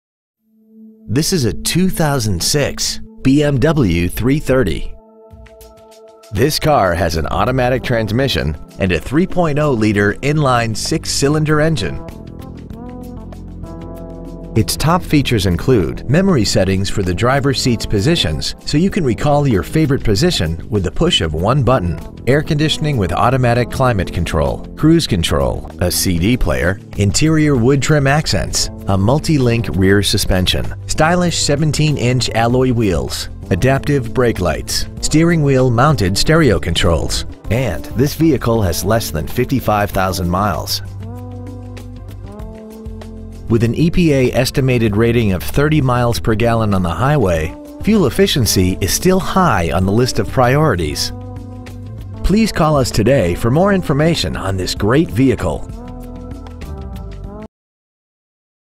2006 BMW 330i Plano TX
Year : 2006
 Make : BMW
 Model : 330i
 Engine : 3.0L DOHC high-output 24-valve I6 engine
 Trans . : Automatic
 Exterior : Gray
 Miles : 54,700
 Interior : Black